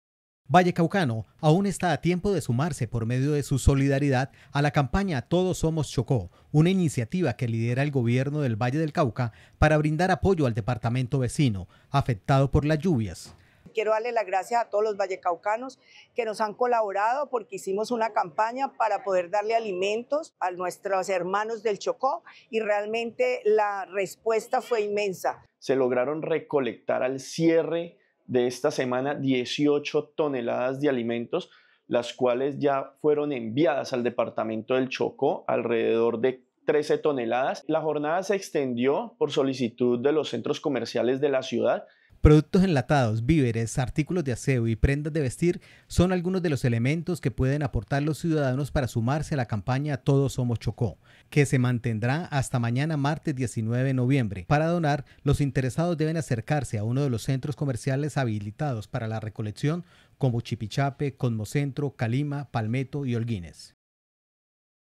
Más de 18 toneladas de ayudas humanitarias han sido recolectadas en el Valle para el Chocó
La campaña Todos Somos Chocó, liderada por el Gobierno del Valle del Cauca, continuará hasta este martes 19 de noviembre en los centros comerciales habilitados para la recolección: Chipichape, Cosmocentro, Calima, Palmetto y Holguines.